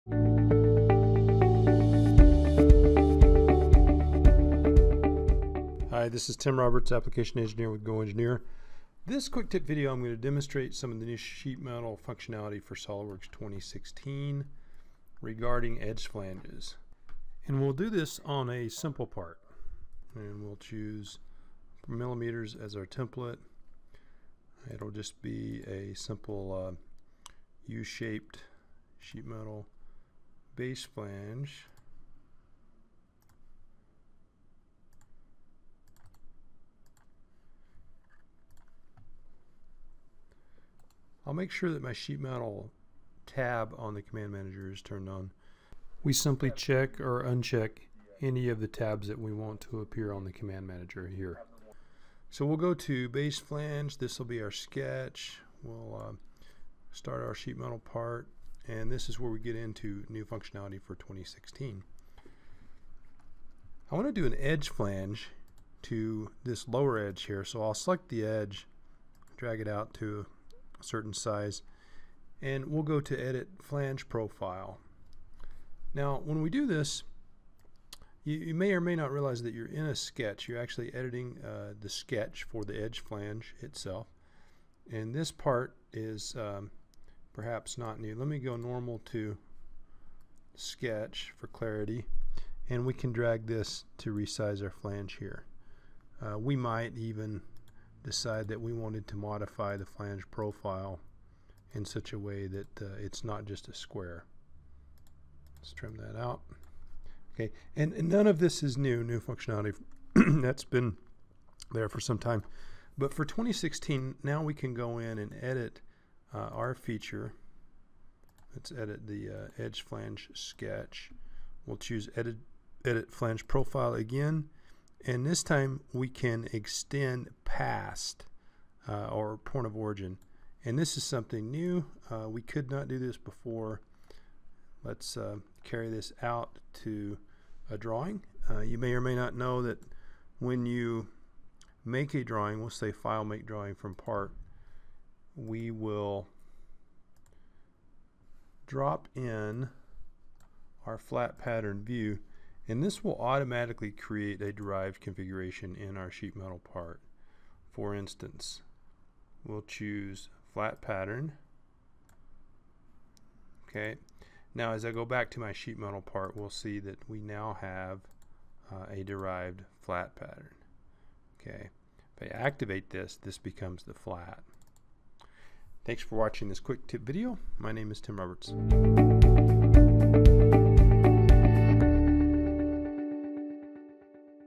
SOLIDWORKS 2016 - Sheetmetal Edge Flange Extension
Learn about the ability to extend an edge flange past its original edge length in SOLIDWORKS 2016 for sheetmetal parts.  Quick Tip presented by Tim Roberts of GoEngineer.